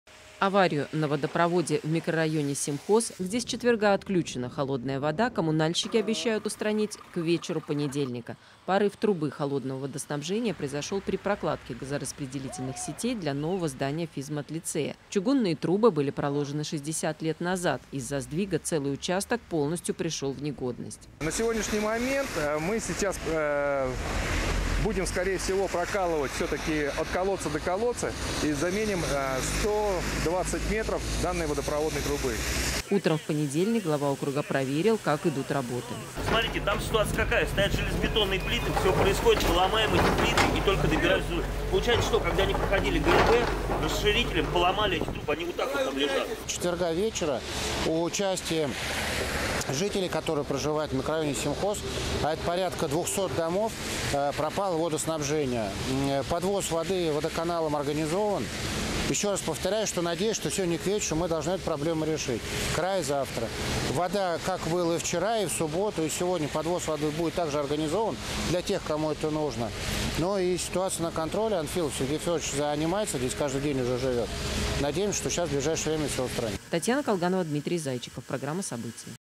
Устранение аварии на контроле
В Семхозе продолжаются работы по восстановлению холодного водоснабжения.